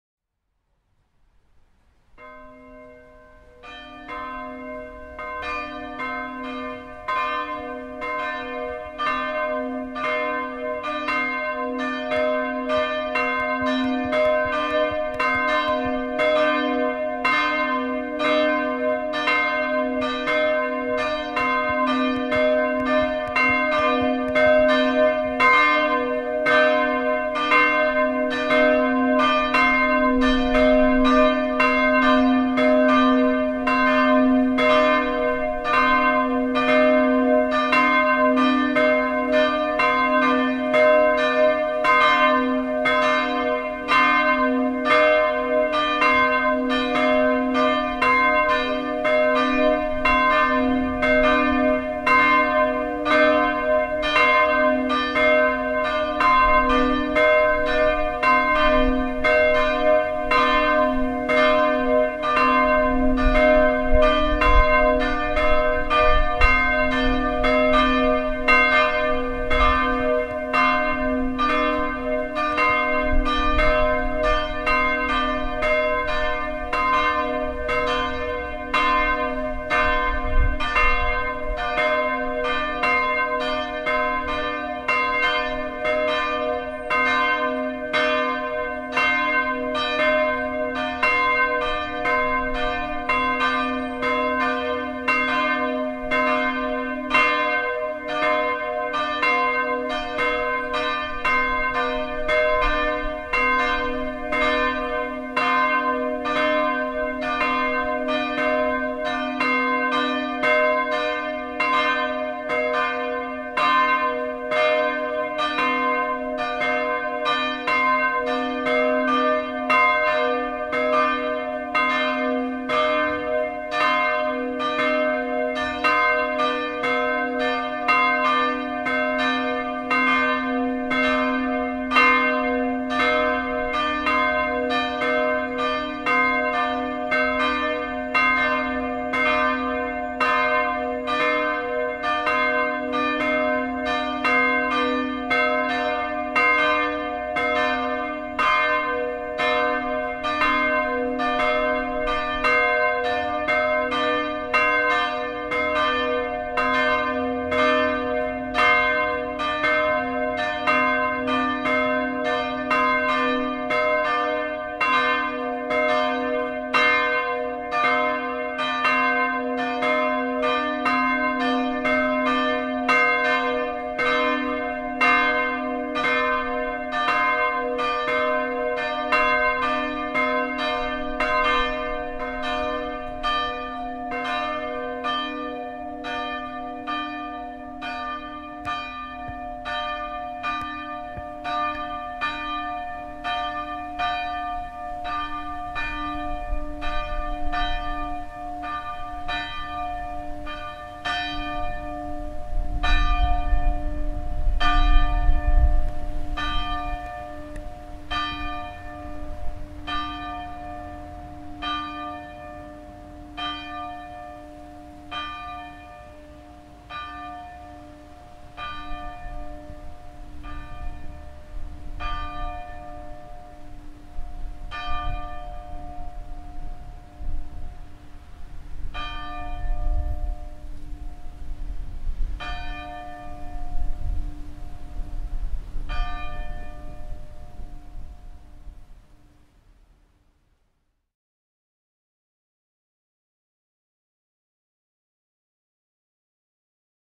Dzwony na Anioł Pański kościoła pw. św. Zygmunta w Szydłowcu
O godzinie 12:00 odzywają się dwa dzwony. Mają tony d/2 oraz c/2 mocno zaniżone, mogą ważyć około 200 i 300kg.